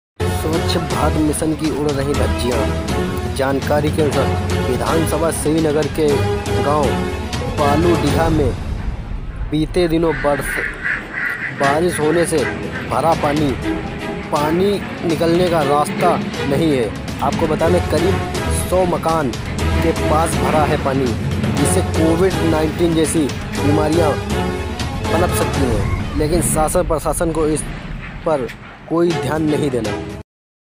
लखीमपुर खीरी सदर तहसील की ग्राम पंचायत बालूडीहा बारिश होते ही रास्ते में बंद
लखीमपुर खीरी सदर तहसील की ग्राम पंचायत बालूडीहा
मैं बारिश होते ही रास्ते हो जाते हैं जल  मांगना